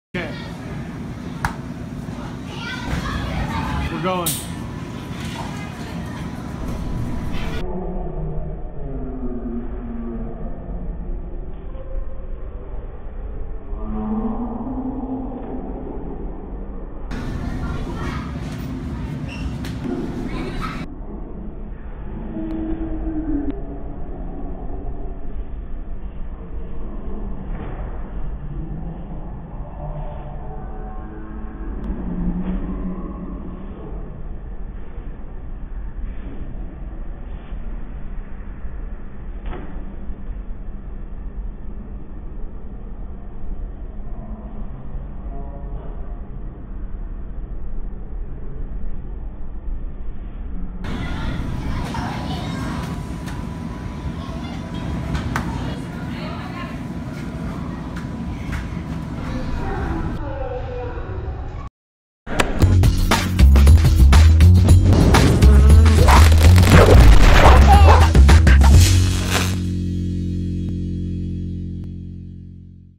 Wow!! Check Out This Guy! He Can Really Spin A Pizza!!
Behind the back, over the shoulder, WOW! Pizza Spinning looks like a lot of fun!